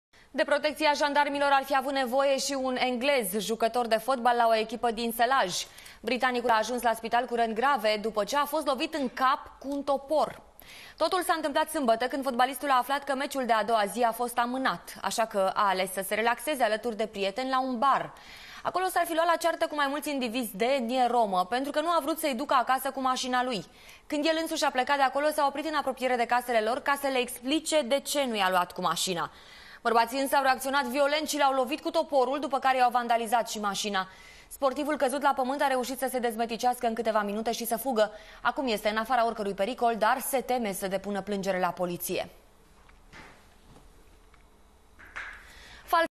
Fotbalist englez, atacat cu toporul
De protecţia jandarmilor ar fi avut nevoie şi un un englez, jucător de fotbal la o echipă din Sălaj.